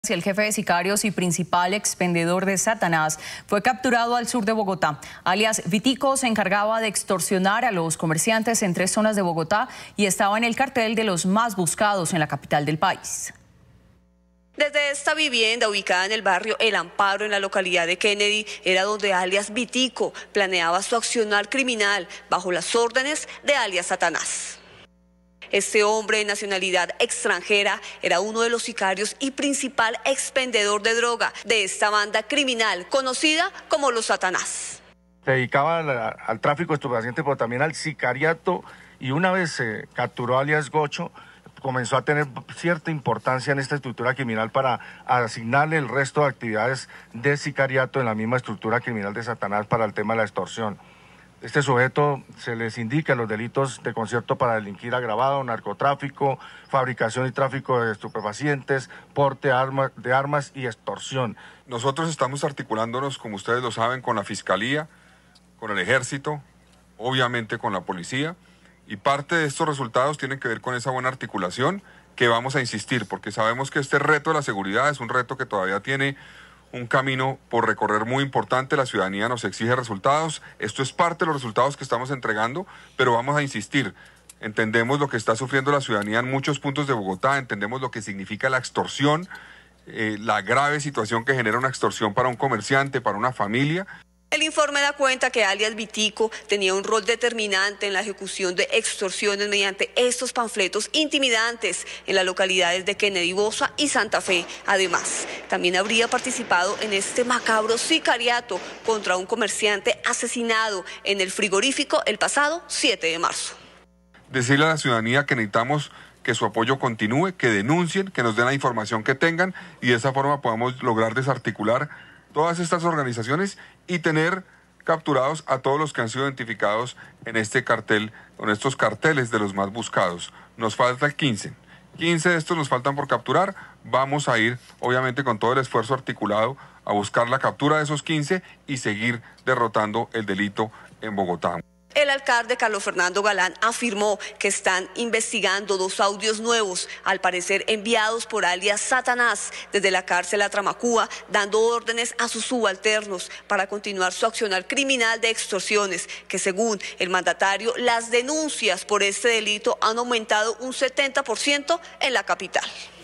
Así cayó alias 'Vitico', uno de los más buscados en Bogotá y principal expendedor de 'Satanás'
El jefe de sicarios y principal expendedor de 'Satanás', fue capturado al sur de Bogotá. Alias Vitico se encargaba de extorsionar a los comerciantes en tres zonas de Bogotá y estaba en el cartel de los más buscados.